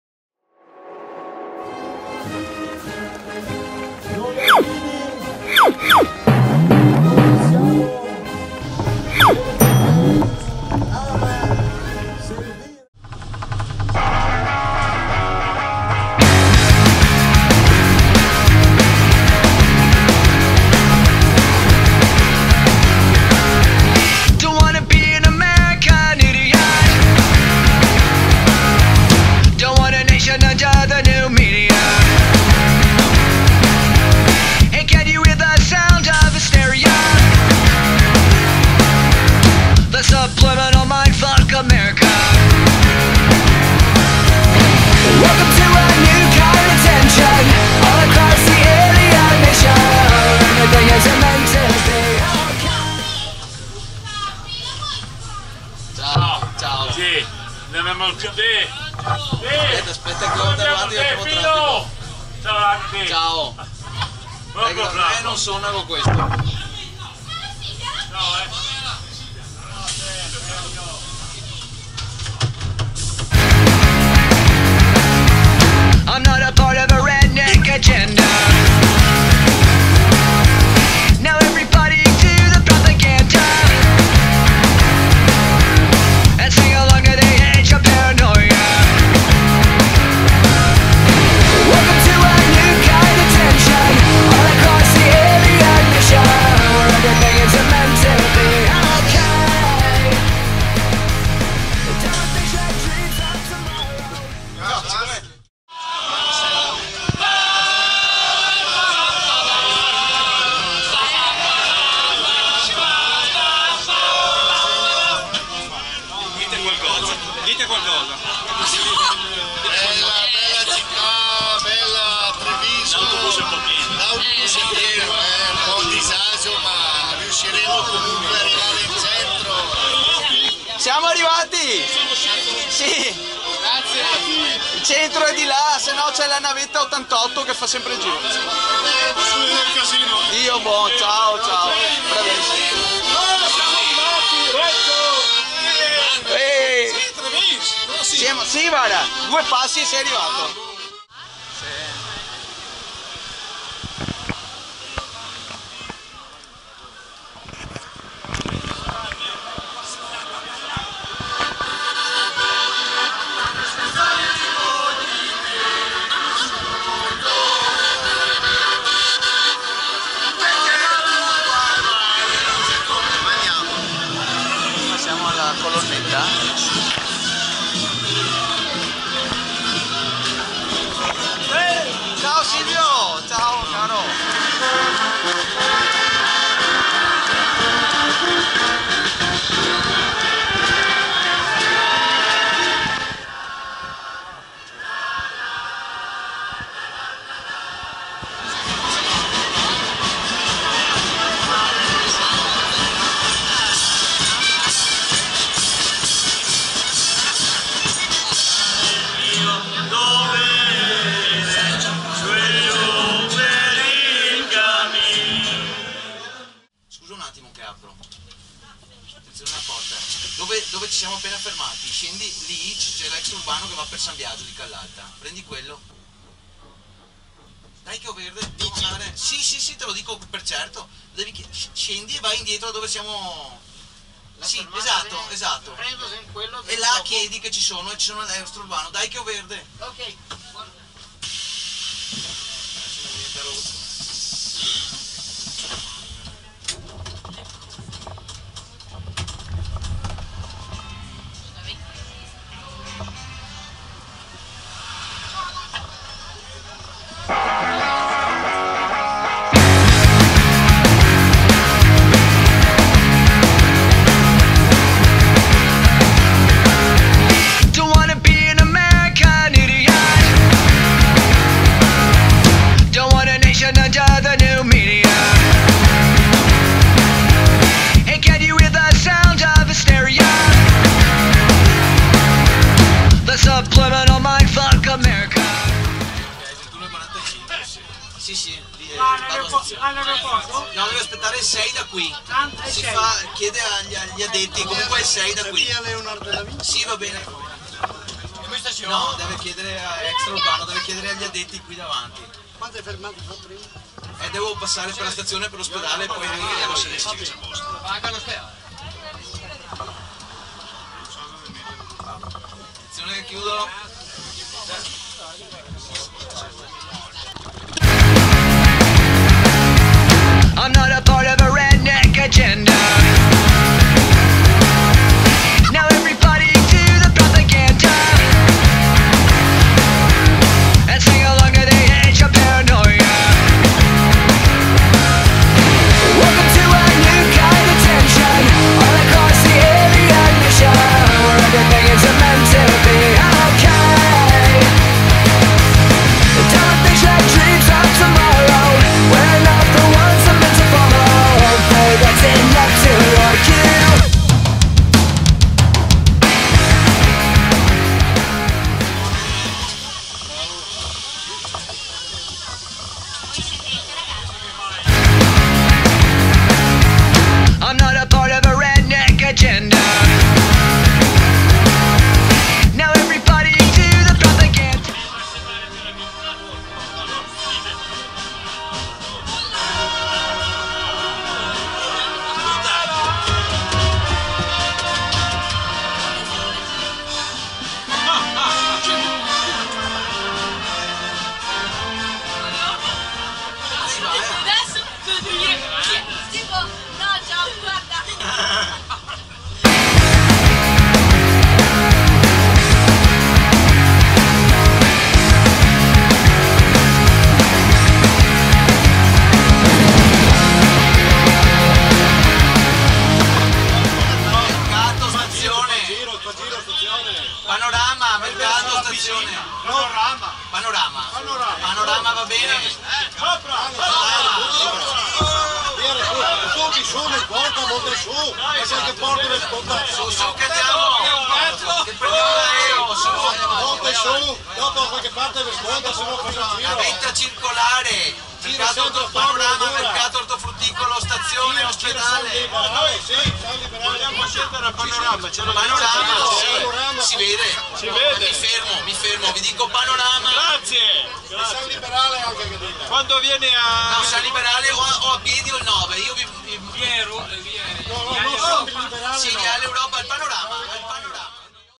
Adunata Alpini Treviso 2017 - Visuale autista
Adunata degli Alpini a Treviso dall'11 al 14 maggio 2017.
Visuale dall'autista dell'autobus